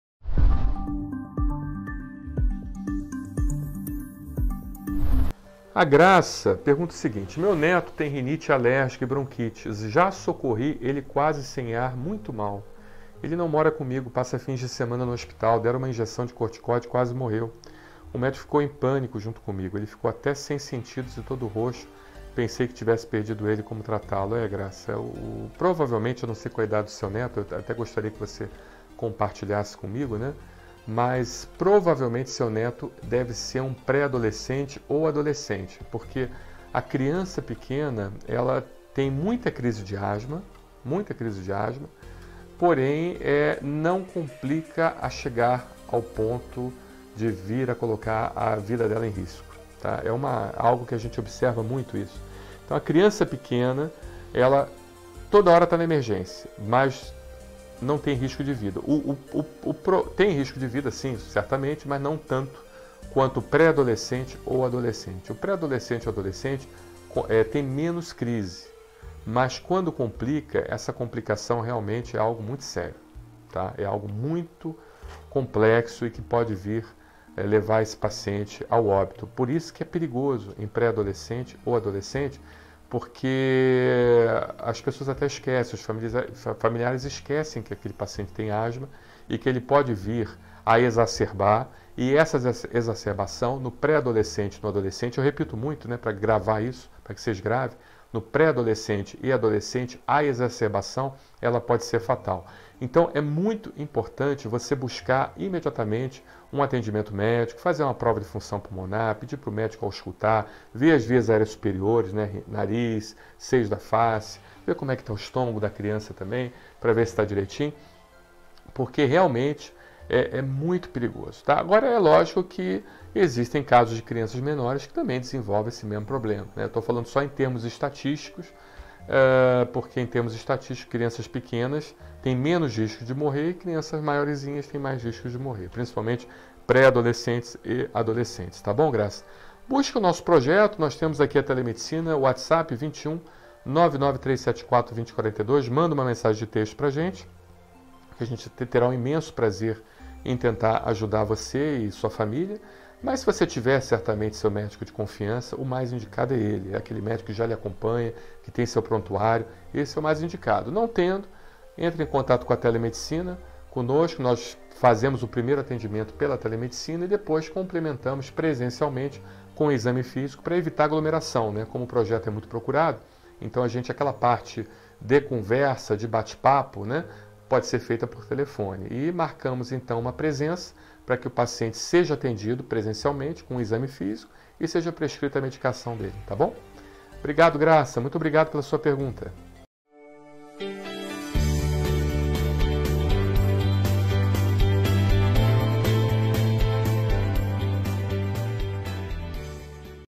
Meu neto tomou injeção de corticoide e passou muito mal, chegou a desmaiar | Como tratar?
Graça- Meu neto tem renite alérgica e bronquite, já socorri ele quase sem ar muito mal, ele não mora comigo passa fins de semana, no hospital deram uma injeção de corticoide e quase morreu, o médico ficou em pânico junto comigo, ele ficou até sem sentidos e todo roxo pensei que tivesse perdido ele. como tratá-lo?